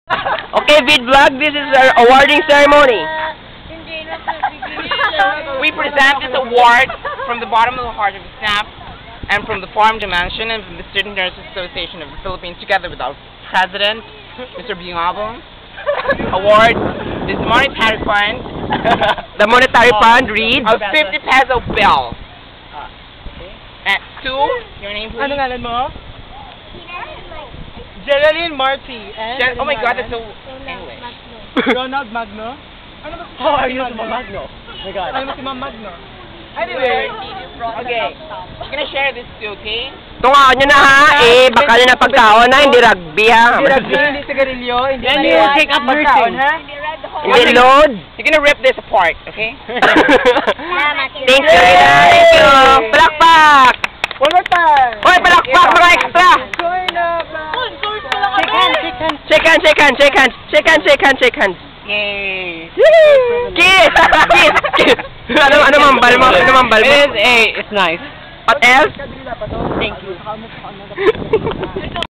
"Awarding" : D
I gave Php30 and Porsha's Php 20 for the kids...Bodo and John held an impromptu awarding ceremony overseen by Lord Bungabong...